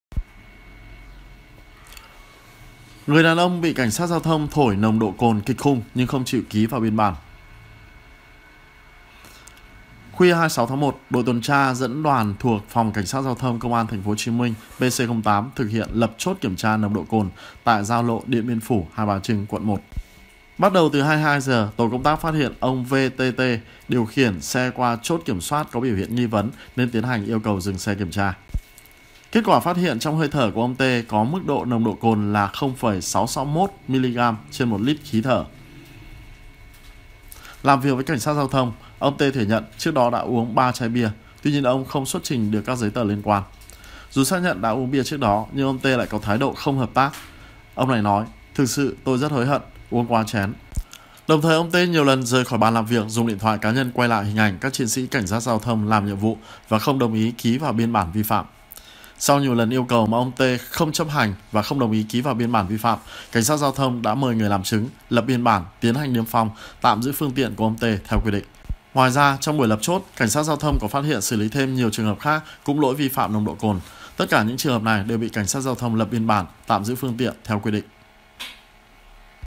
Người đàn ông bị CSGT thổi nồng độ cồn kịch khung nhưng không chịu ký vào biên bản | THVN
Ông V.T.T. điều khiển xe qua chốt kiểm soát, có biểu hiện nghi vấn nên CSGT yêu cầu dừng xe kiểm tra. Kết quả phát hiện trong hơi thở của ông T. có mức nồng độ cồn là 0,661 miligam/lít khí thở, ông này thừa nhận có uống bia, nhưng không ký vào biên bản, liên tục cầm điện thoại quay video.